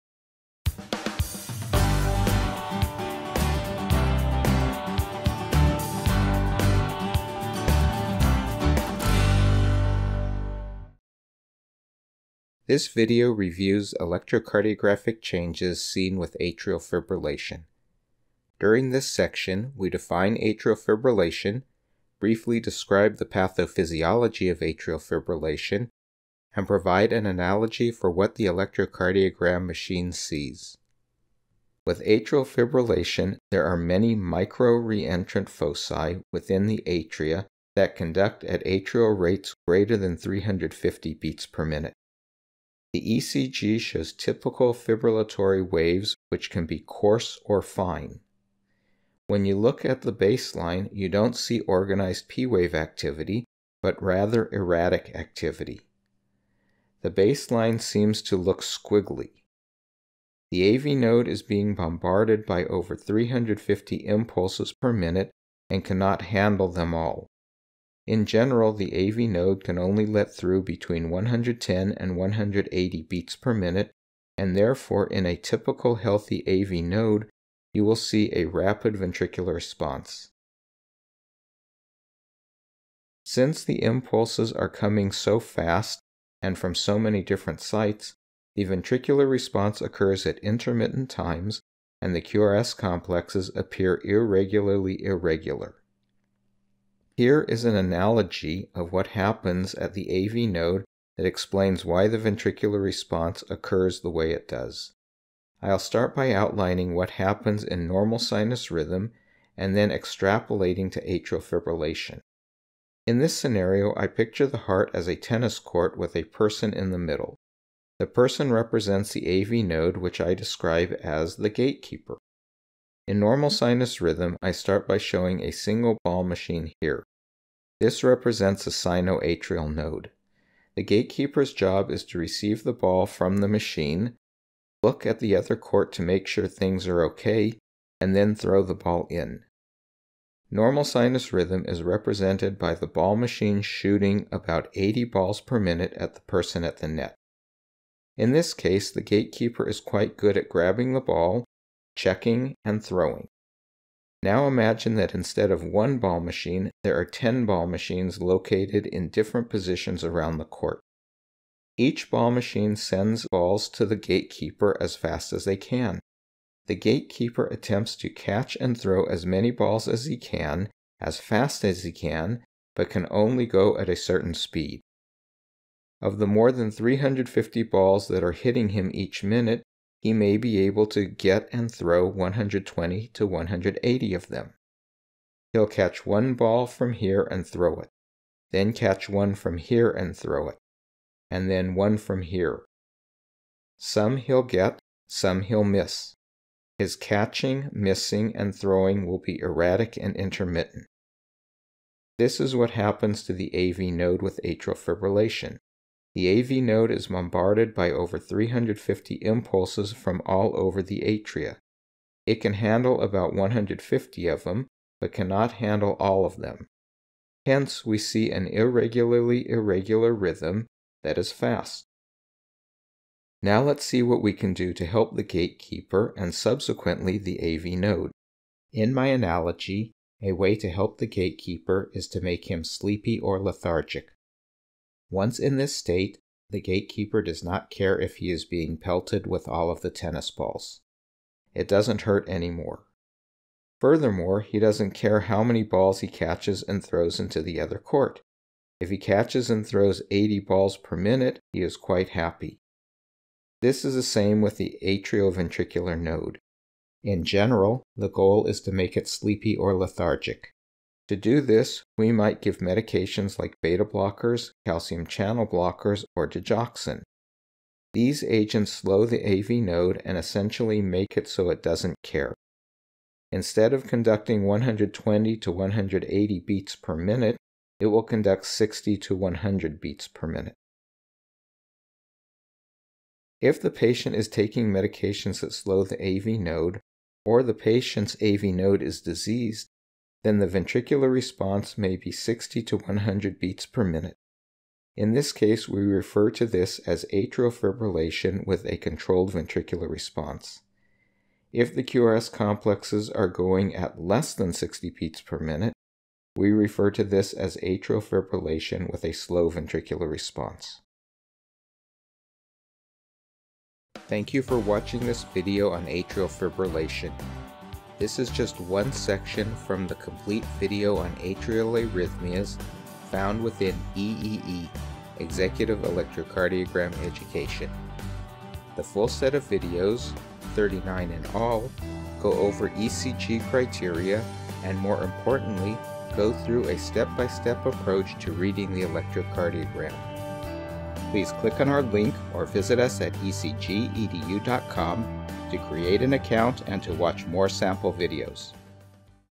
Executive Electrocardiogram Education - Atrial Fibrillation Preview Video
Executive Electrocardiogram Education (EEE) - The Last Course You'll Ever Need To Learn To Read ECGs, Presents This Preview Video On Atrial Fibrillation. This Is Just One Part On An Entire Video About Atrial Arrhythmias. This Video Series Gives Detailed Diagrams And Analogies For ECG Abnormalities.